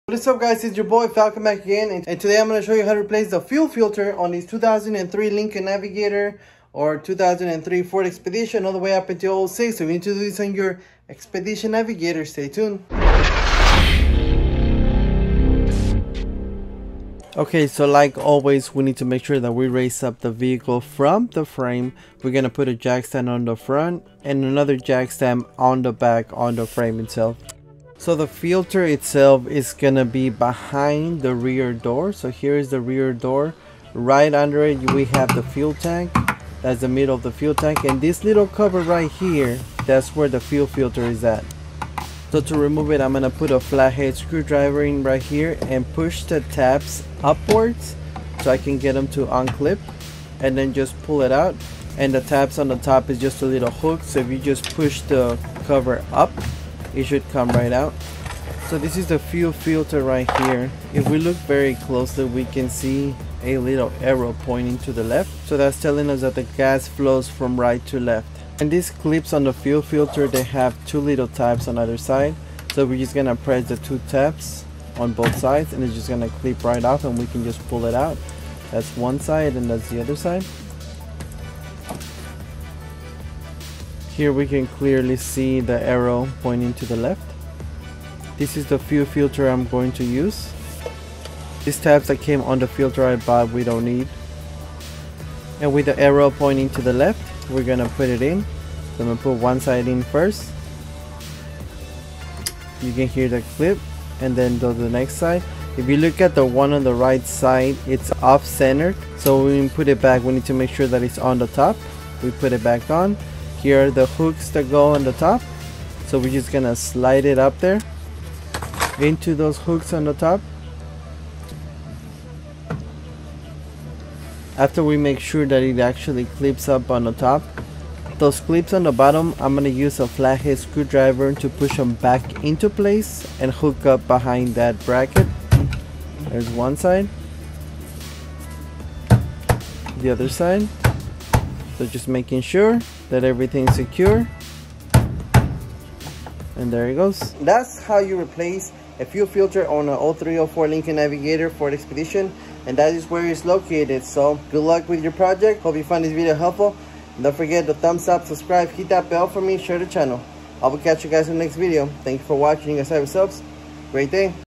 Ford Expedition Lincoln Navigator Fuel Filter Replacement 2002-2004
today i will show you where and how to replace you fuel filter on your 03-04 navigator expedition.

IF you would like to leave a donation, here is my link anything helps